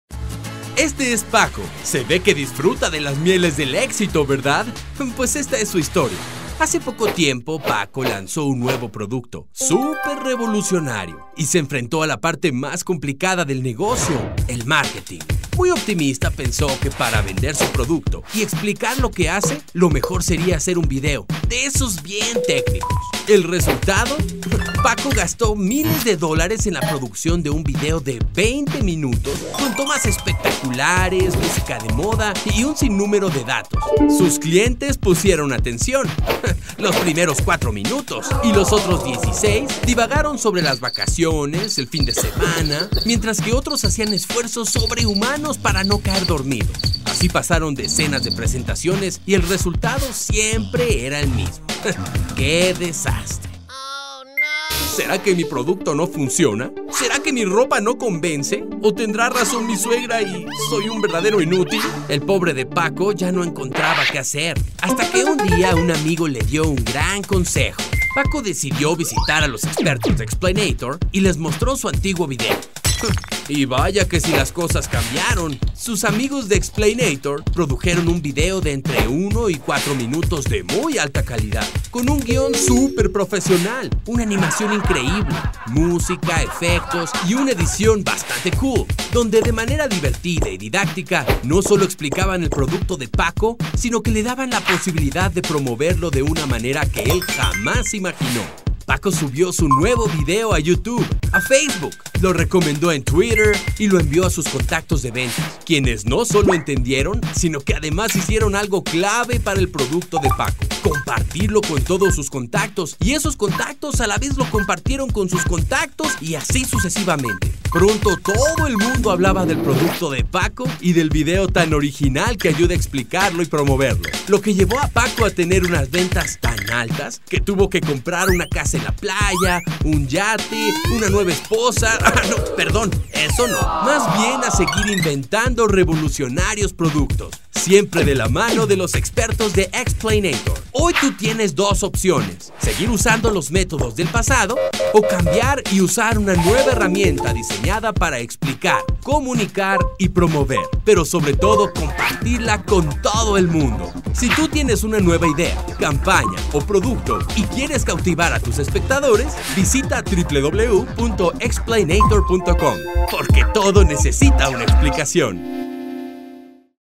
Demo Xplainator Versión Español
Videos producidos por Xplainator con el objetivo de convertirse en Virales al ser compartidos

Xplainator Videos produced to become viral, the whole idea is that people, between 1 and 4 minutes, will be able to understand a product or service through a fun and educational video that ended up being shared with everybody...